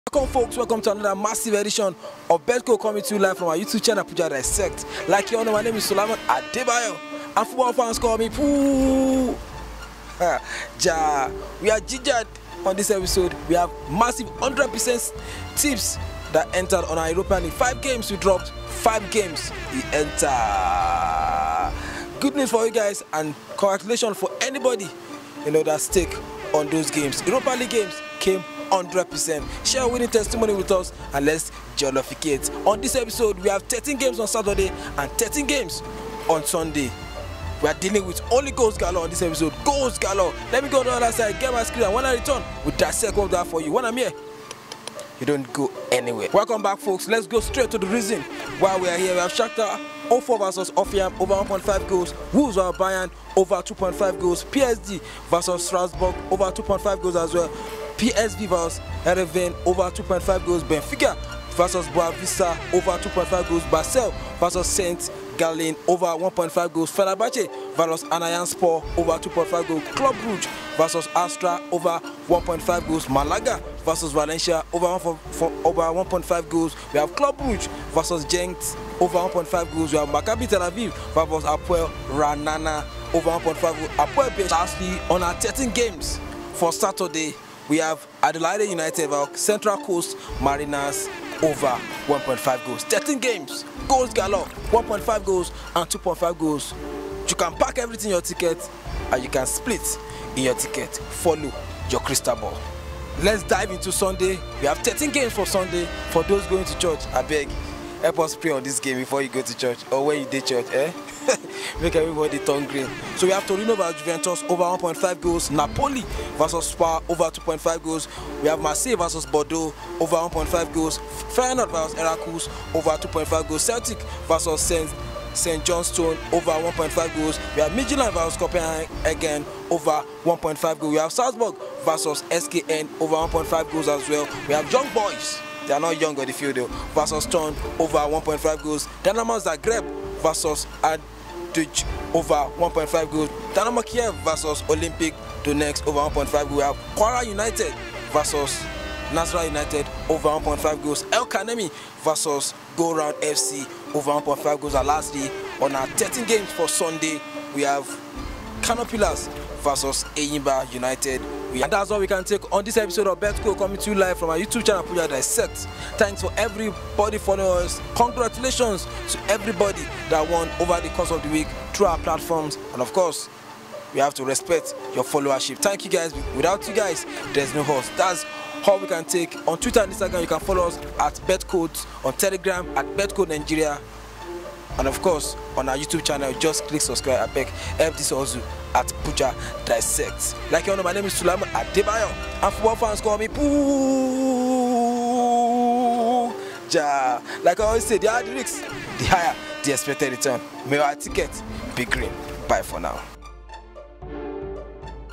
26 Banker Games For This Weekend Matches Across Europe (Season 1 - Episode 23)
26 games to pick from the games across Europe this weekend. Its a pool of a games. Pick yours!